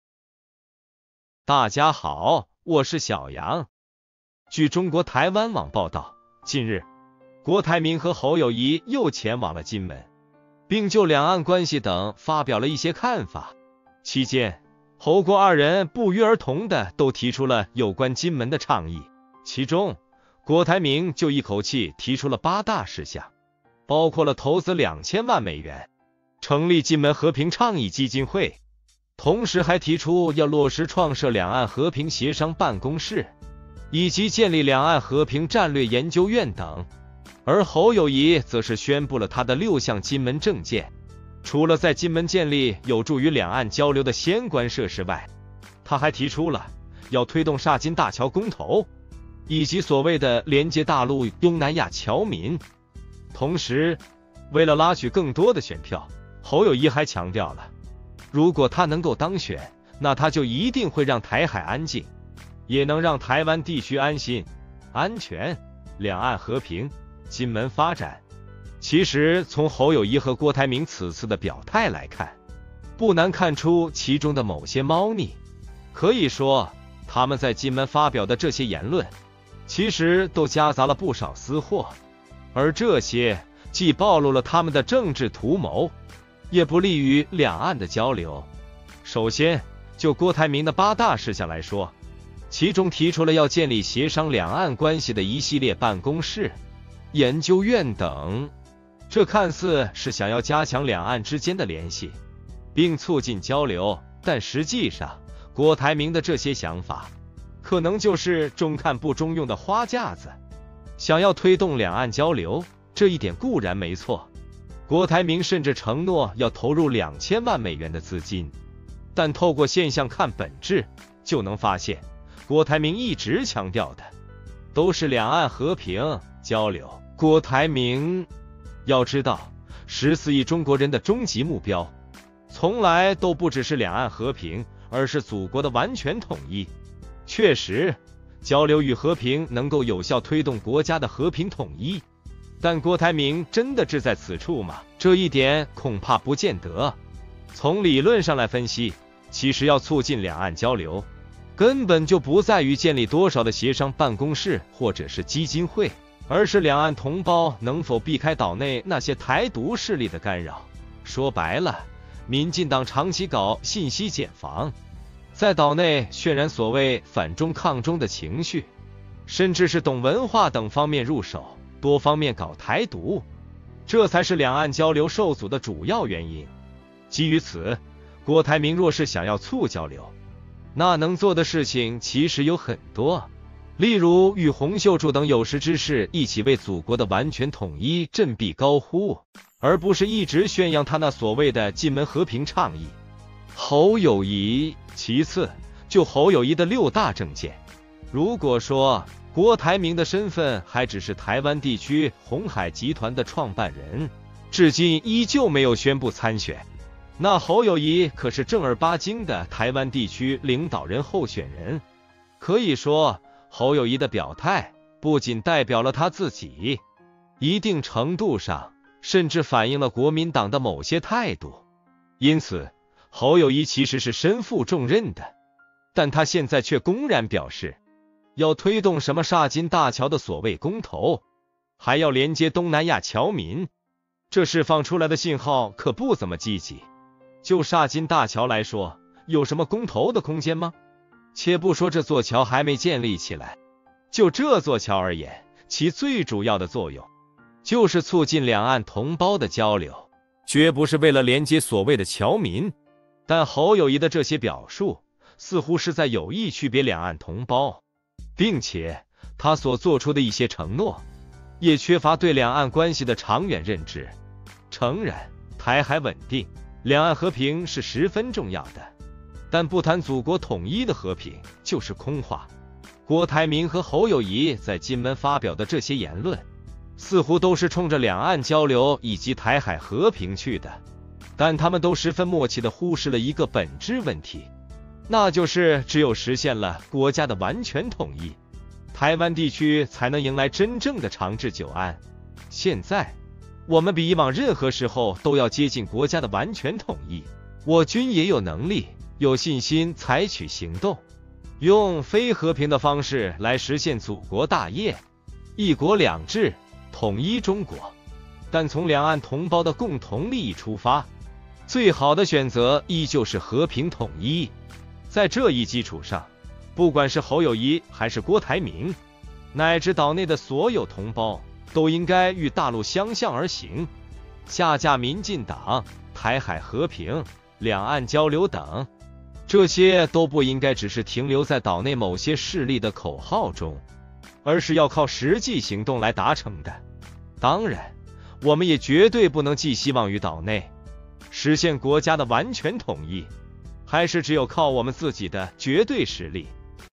郭台铭侯友宜在金门隔空对战，为2024年台湾地区选举造势
微信号：
wangyang14121
推特账号：wangyang14121